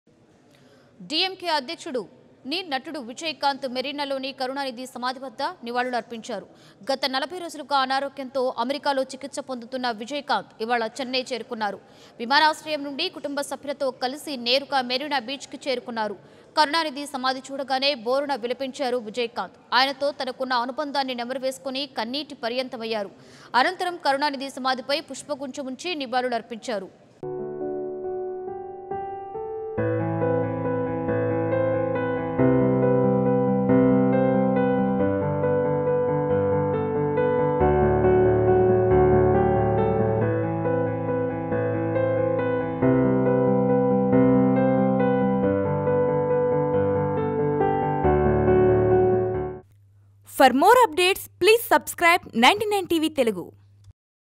Hero Vijayakanth Get Emotional at Karunanidhi Memorial | Kalaignar | 99 TV Telugu
Tamil Hero Vijayakanth Get Emotional at Kalaignar Karunanidhi Memorial